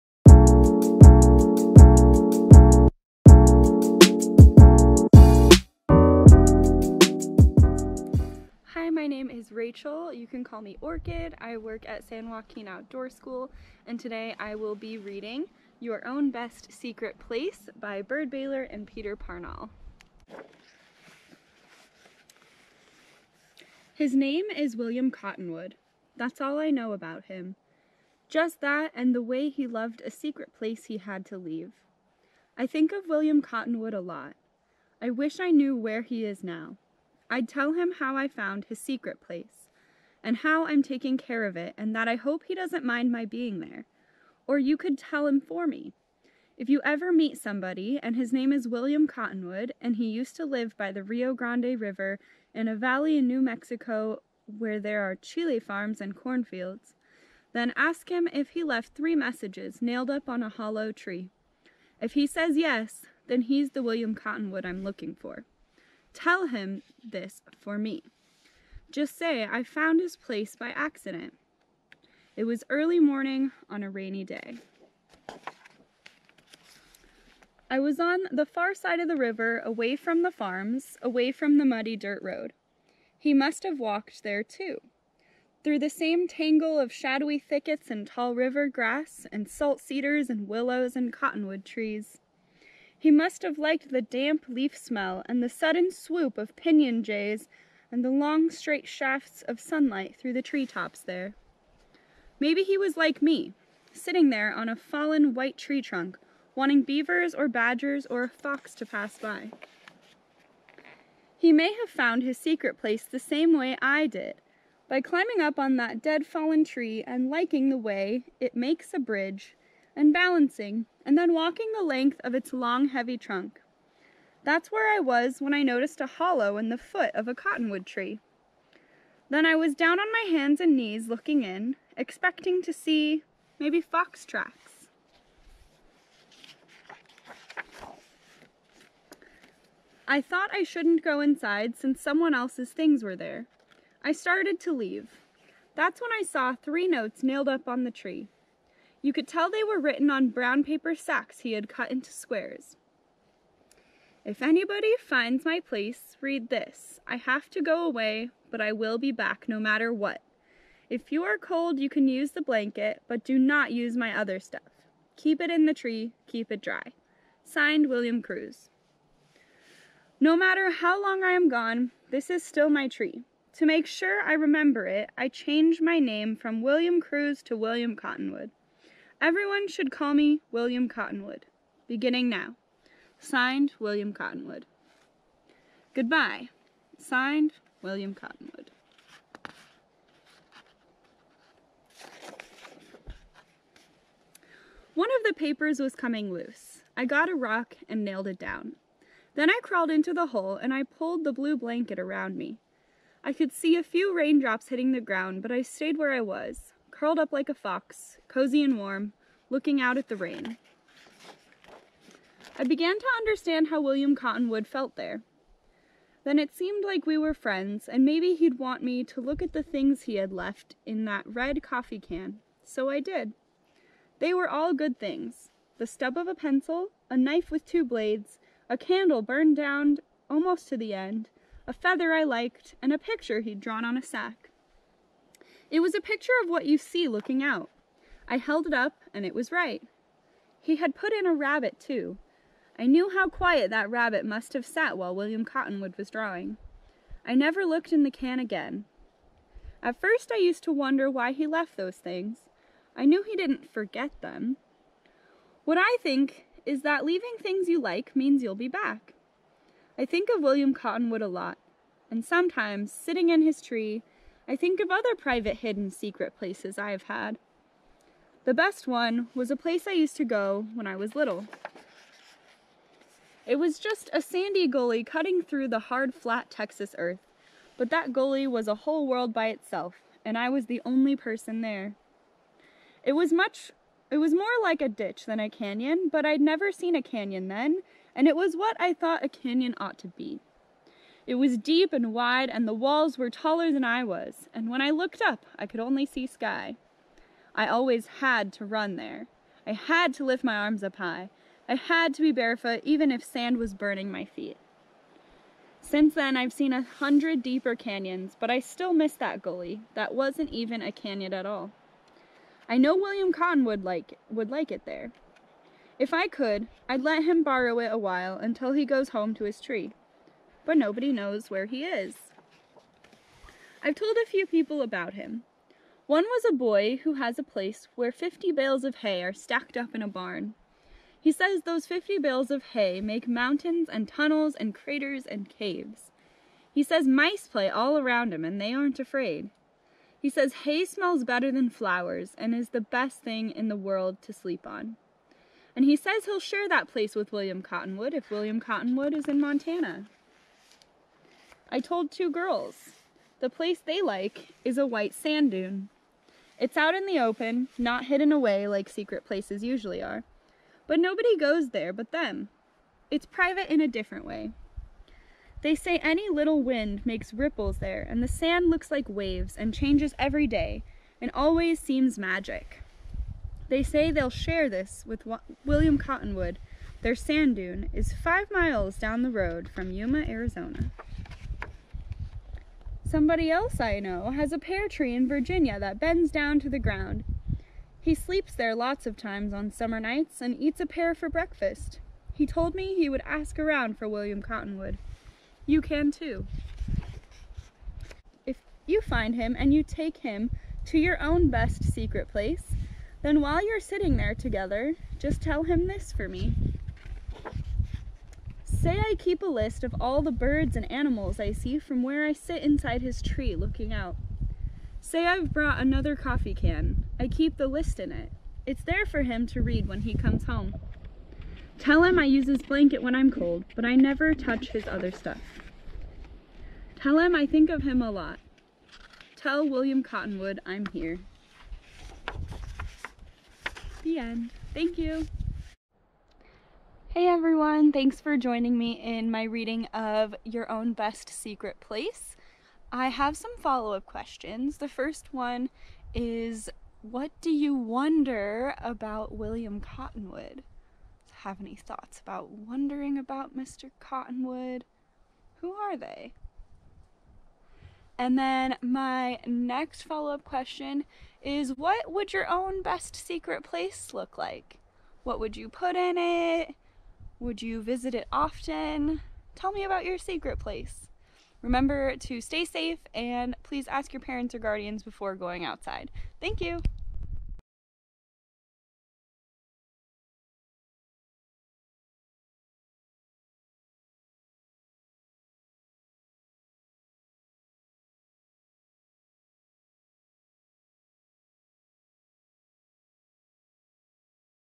Your Own Secret Place with Orchid
Join our Naturalist Orchid from SJCOE Outdoor Education to hear her read Your Own Best Secret Place by Byrd Baylor. You will also see many of the coastal redwood trees located at Camp Jones Gulch.